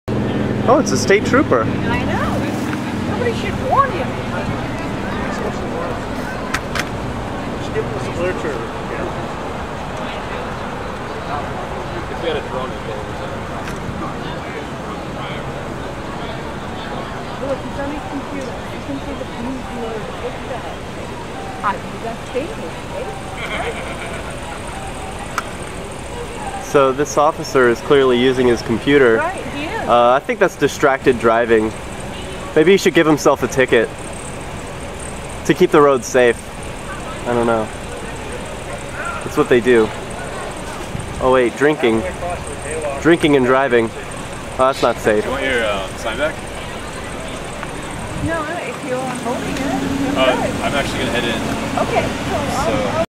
Unsafe: Distracted State Trooper Uses Laptop in Car | Neutralizing a DUI Checkpoint Part 16
Friday, July 11th, 2014 - Activists in Manchester noticed that the police announced a checkpoint for DUIs for tonight, so dozens of activists joined in holding signs to direct drivers away from the DUI checkpoint.